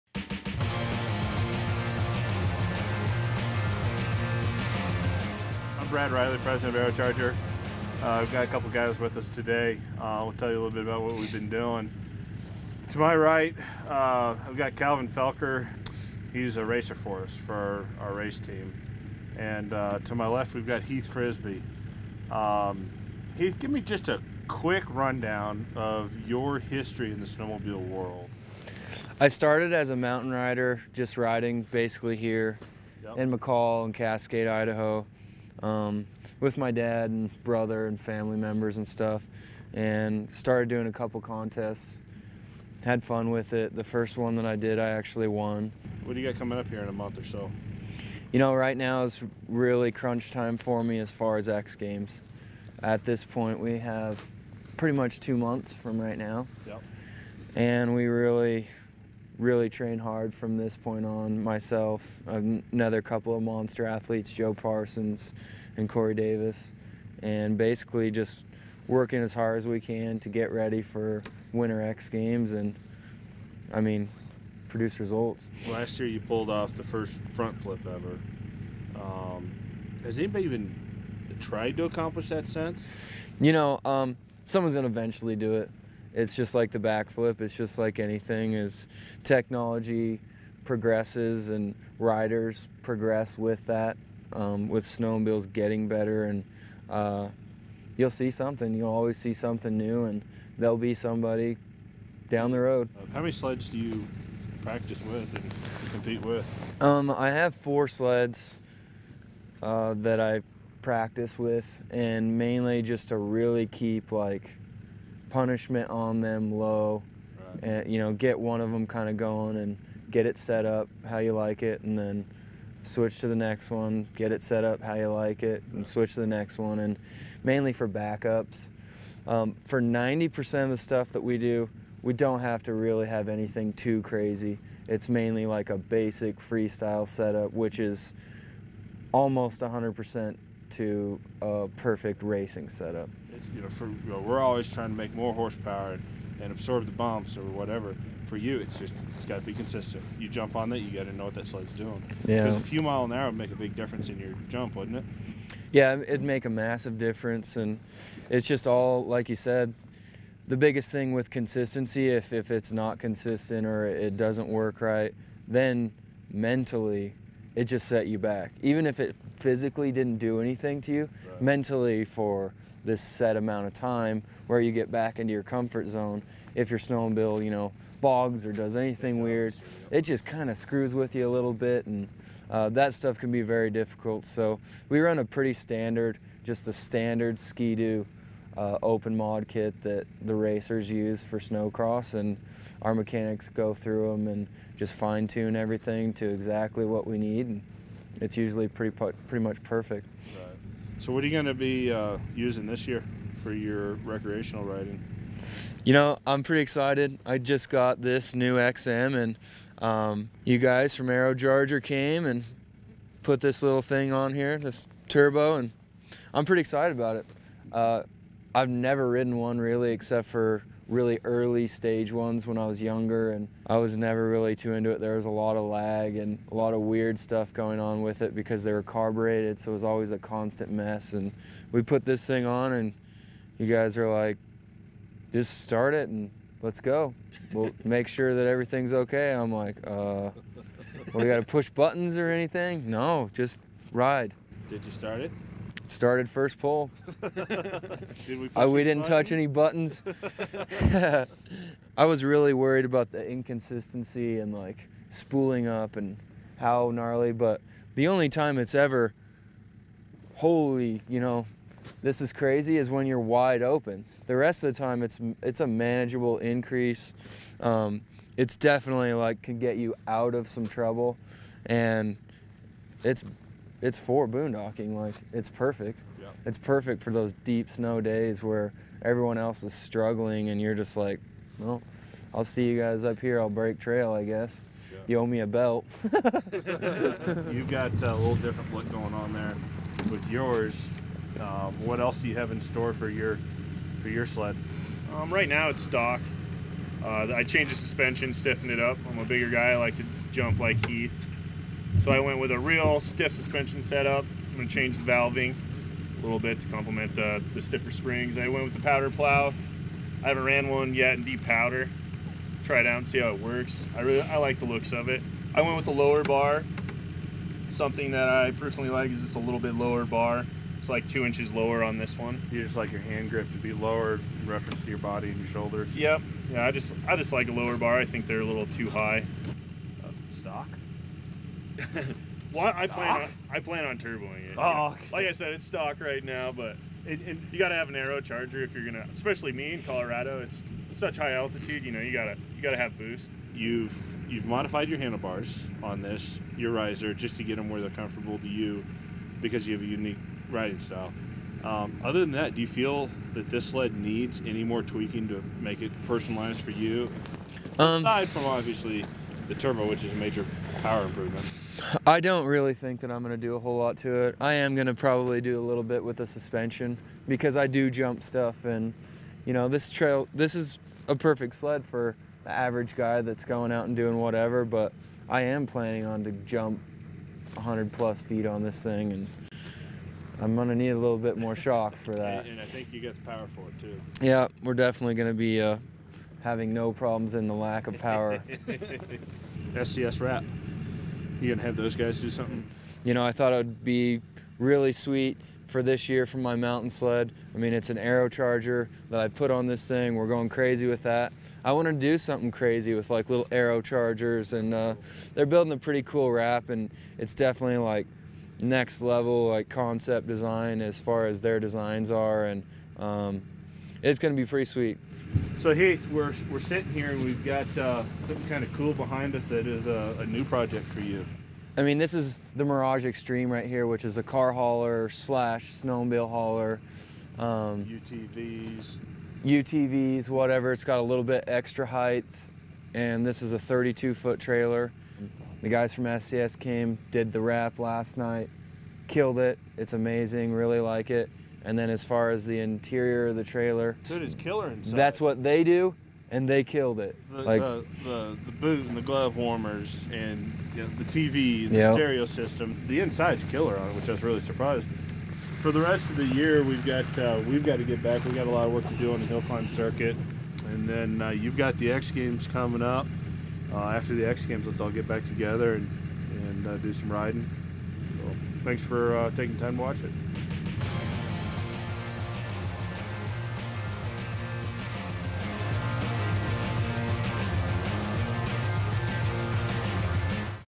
Heath Frisby - McCall Idaho Snowmobile Interview
Heath and Brad hang out in a snowy parking lot in Idaho and talk about turbochargers, shocks, bars, wraps, and launching sleds through the air.